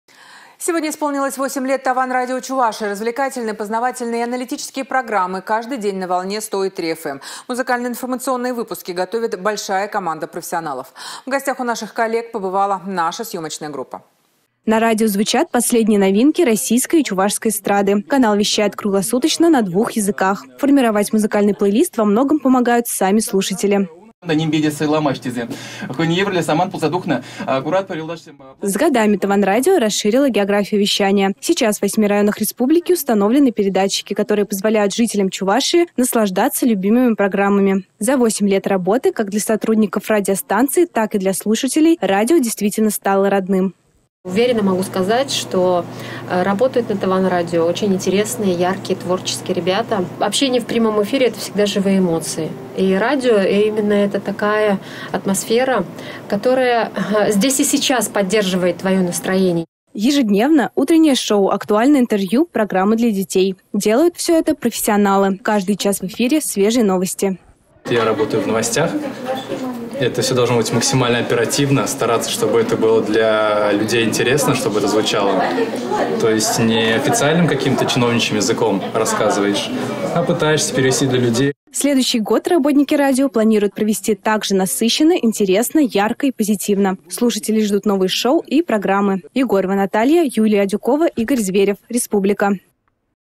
Восьмилетие «Тăван радио»
Сегодня исполнилось восемь лет «Тăван радио» Чувашии. Развлекательные, познавательные и аналитические программы – каждый день на волне 100,3 ФМ. Музыкально-информационные выпуски готовит большая команда профессионалов. В гостях у наших коллег побывала наша съемочная группа. На радио звучат последние новинки российской и чувашской эстрады. Канал вещает круглосуточно на двух языках. Формировать музыкальный плэйлист во многом помогают сами слушатели. С годами «Тăван радио» расширило географию вещания. Сейчас в восьми районах республики установлены передатчики, которые позволяют жителям Чувашии наслаждаться любимыми программами. За восемь лет работы, как для сотрудников радиостанции, так и для слушателей радио действительно стало родным. НАДЕЖДА АНДРЕЕВА: "Уверенно могу сказать, что работают на «Тăван радио» очень интересные, яркие, творческие ребята. Общение в прямом эфире это всегда живые эмоции. Радио - это такая атмосфера, которая здесь и сейчас поддерживает твое настроение". Ежедневно - утреннее шоу, актуальные интервью, программы для детей. Делают все это профессионалы. Каждый час в эфире - свежие новости. ДМИТРИЙ ПУГАНОВ: «Я работаю в новостях. Это все должно быть максимально оперативно, стараться, чтобы это было для людей интересно, чтобы звучало, то есть не официальным чиновничьим языком рассказываешь, а пытаешься перевести для людей». Следующий год работники радио планируют провести так же насыщенно, интересно, ярко и позитивно. Слушателей ждут новые шоу и программы